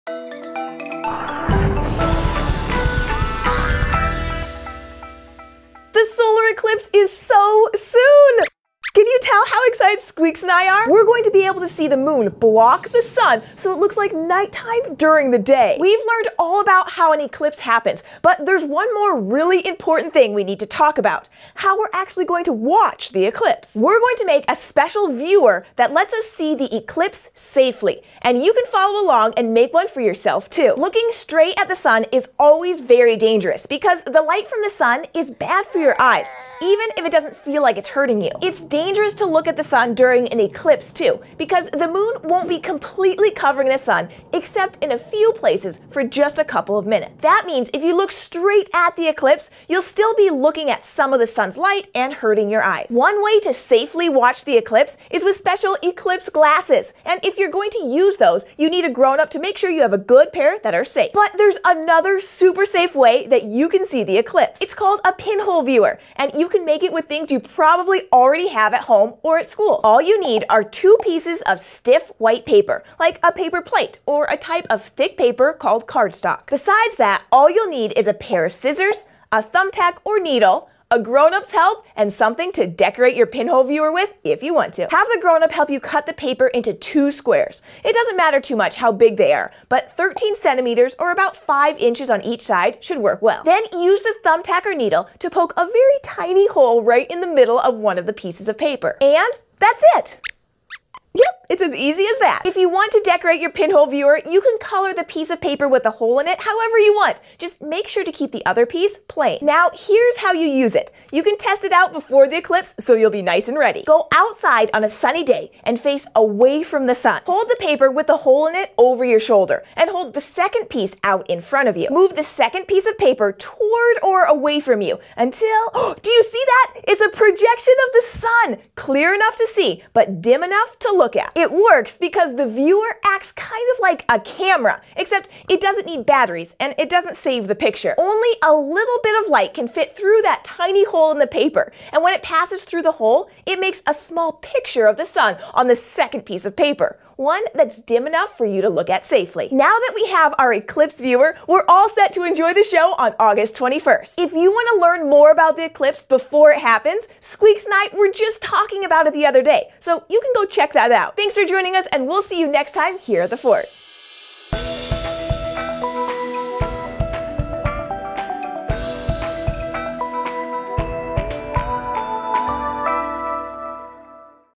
Make an Eclipse Viewer! | Science Project for Kids
Jessi and Squeaks show you how to make a pinhole viewer so you can safely watch the solar eclipse!